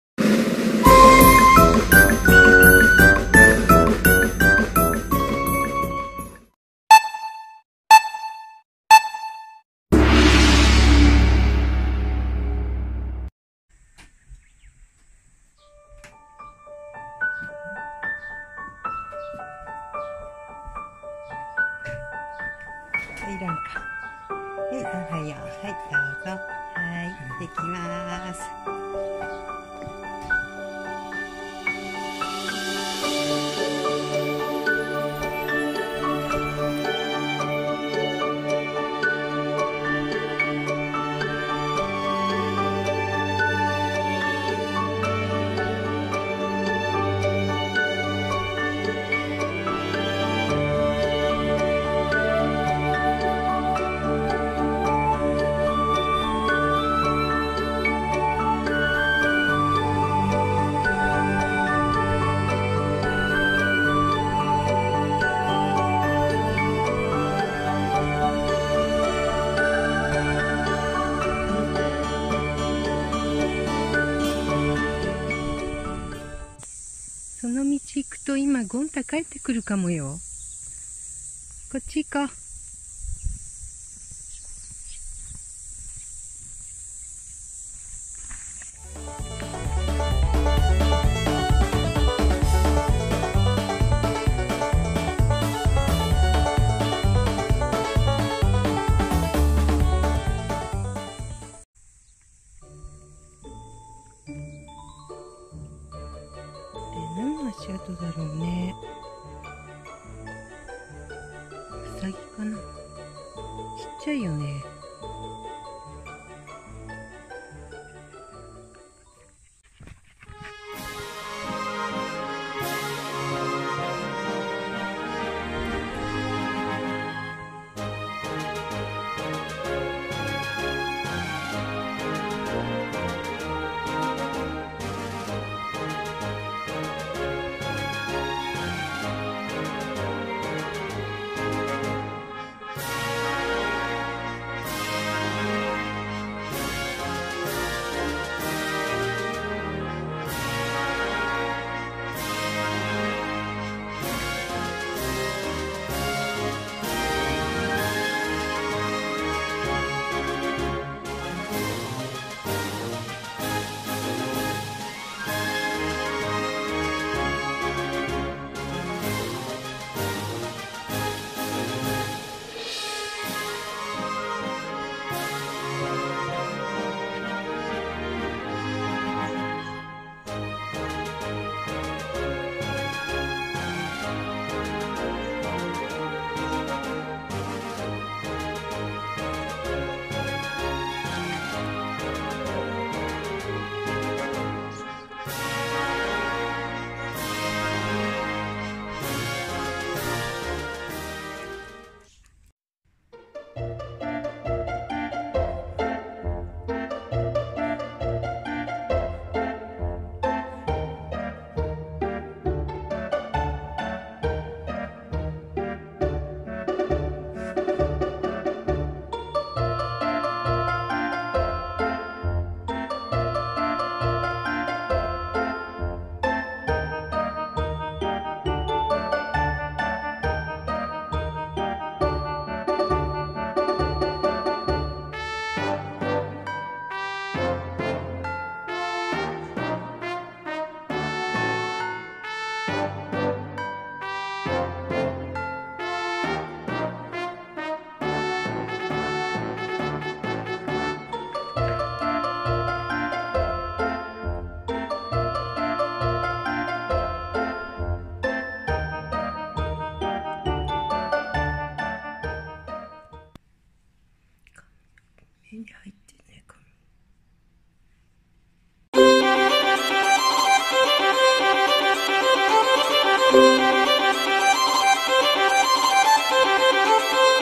2018/7/18【リカオン毎朝動画▪お散歩とぬいぐるみ】
お散歩から帰り、涼しいお部屋でぬいぐるみで遊んだよ😃